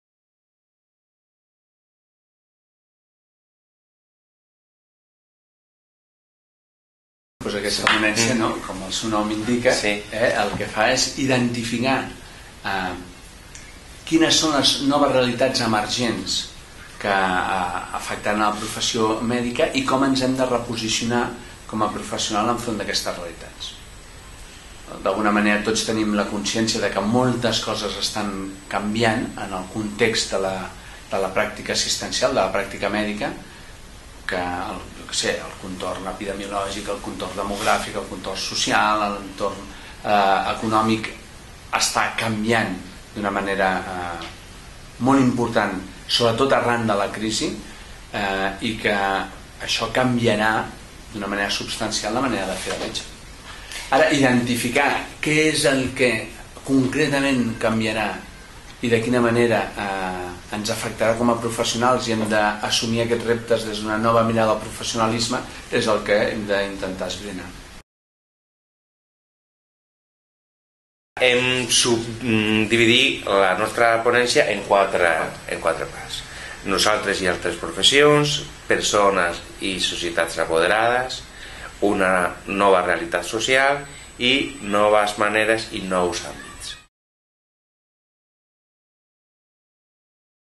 Ponència 2: Quins són els reptes del professionalisme?
L'objectiu de la ponència número 2 del 3rCPMC és identificar quines són les noves realitats emergents que afecten a la professió mèdica i analitzar de quina manera s'haurà de reposicionar per fer front a aquests canvis en les properes dècades. Els doctors Carles Blay i Francisco Garcia, coordinadors d'aquesta ponència expliquen que les noves generacions de metges i metgesses "haurien d'estar-ne amatents, per tal de garantir una medicina de qualitat actualitzada i mantenir la influència social de la professió". A més, en el document es descriu el perfil del metge/metgessa que la ciutadania catalana necessita.

El grup redactor ha subdividit la ponència en quatre apartats que posen en relació la professió mèdic amb altres professions, amb societats apoderades, amb les noves realitats socials i amb els nous àmbits d'actuació que es presenten. Si aquest tema és el que més us motiva i voleu dir la vostra, podeu participar en el debat de forma molt senzilla: registrant-vos en aquest espai que estarà obert fins el dia 31 de juliol.